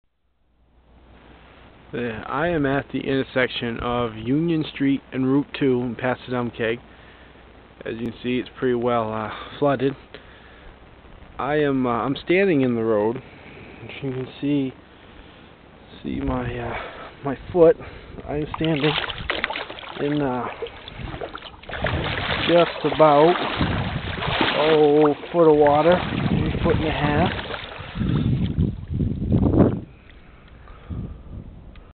Route 2 and Union Flooding
The flooding in the spring of 2008 in passadumkeag, maine.  The penobscot river flooded on April 30th through May 2ec.